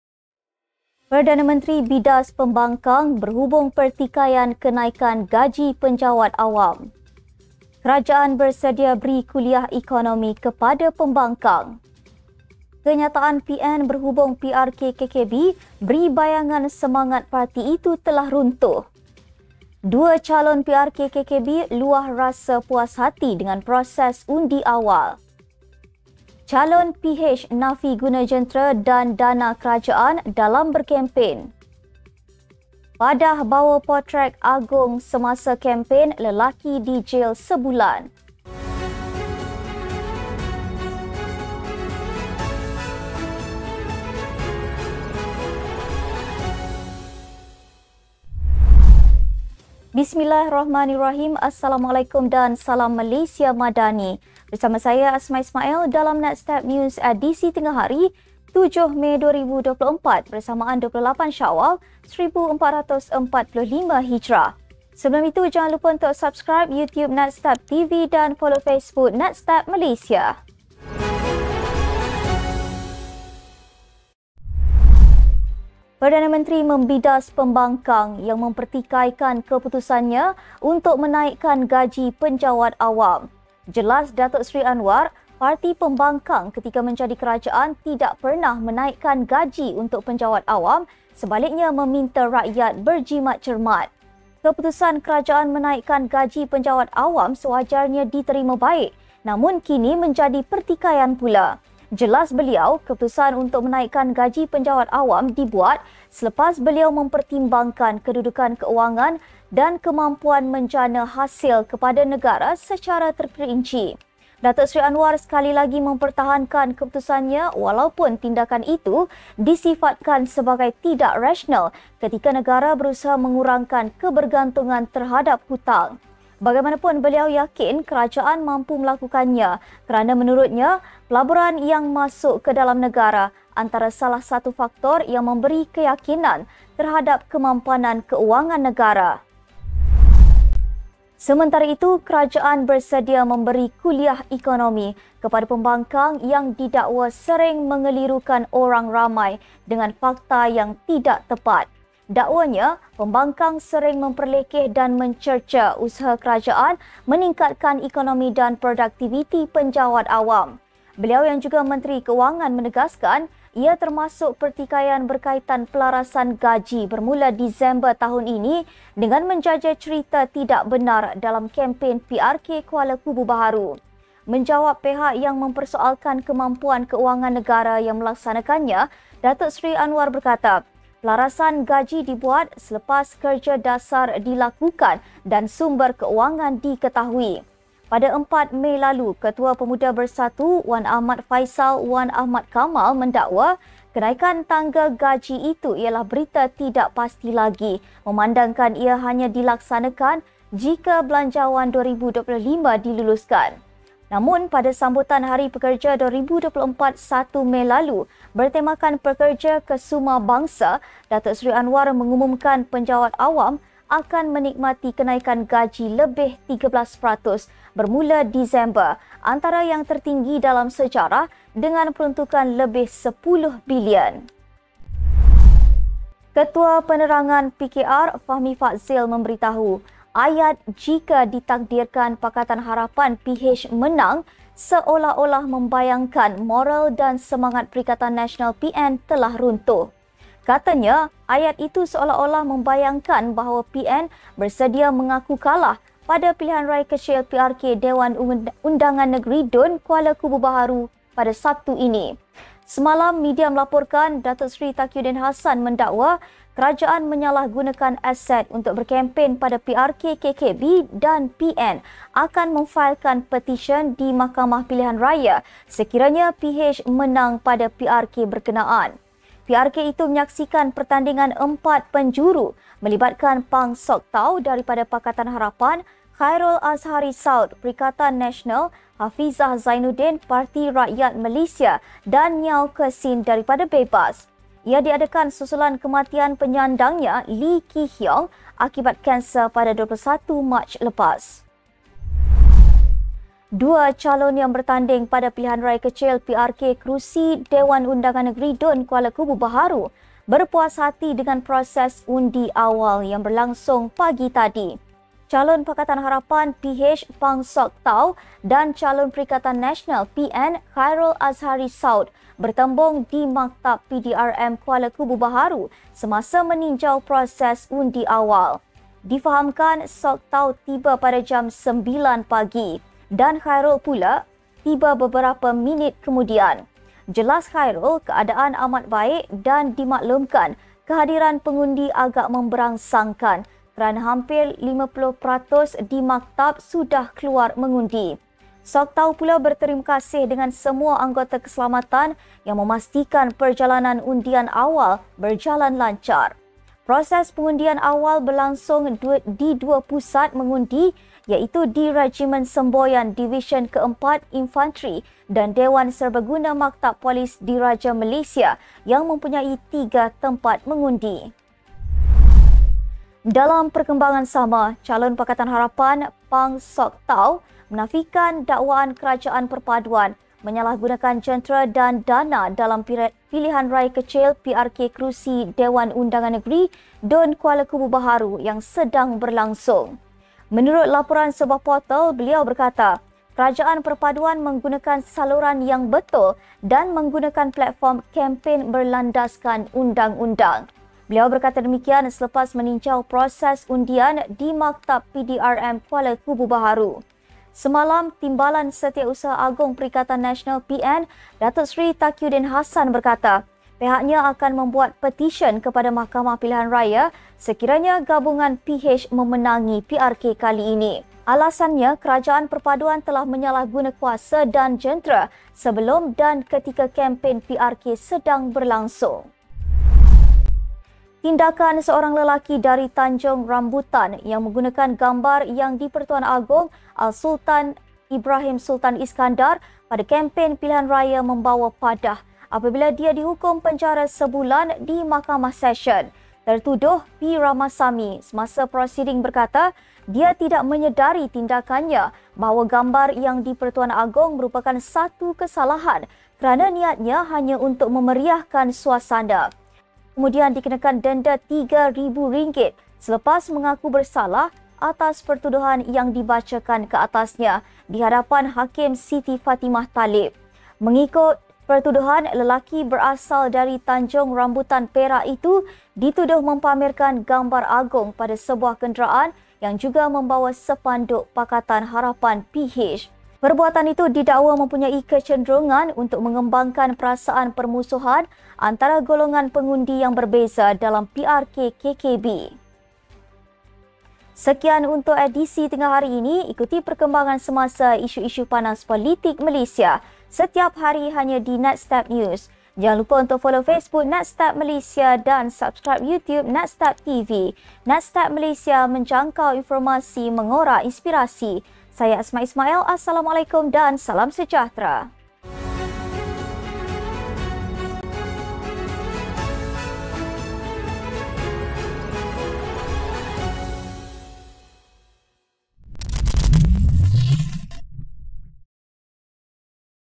PANAS! PN beri 'bayangan' kekalahan - Fahmi | Pembangkang perlu kuliah ekonomi- PMX
Antara yang menarik tengahari ini;

1. “Masa perintah potong elaun, suruh ikat perut, sekarang gaji naik marah” – PM
2. Kerajaan sedia beri ‘kuliah ekonomi’ kepada pembangkang, kata PM
3. 'Ayat jika PH ditakdir menang bayangkan semangat PN runtuh'
4. Dua calon PRK KKB puas hati proses undi awal
5. Sock Tao nafi PH guna jentera, dana kerajaan dalam kempen
6. PRK KKB: Salah guna gambar Agong, Ramasamy dijel sebulan